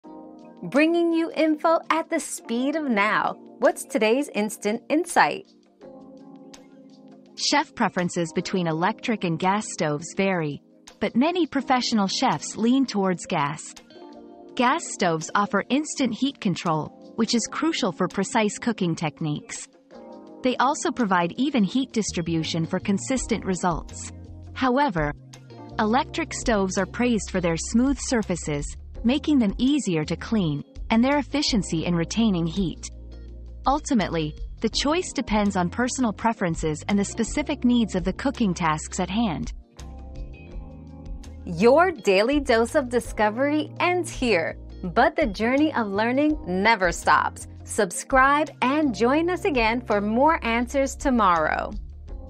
Do chefs prefer electric or gas?
Gas vs Electric: What do chefs prefer? 👉 Gas vs Electric 👉 Discover the preferences of professional chefs when it comes to gas and electric stoves. Find out the advantages of each and how they impact cooking techniques and results. Whether it's instant heat control or easy cleaning, learn which option suits your needs in the kitchen.

Laura S. Harris (2024, January 27.) Do chefs prefer electric or gas?
    🌐 AskAbout.video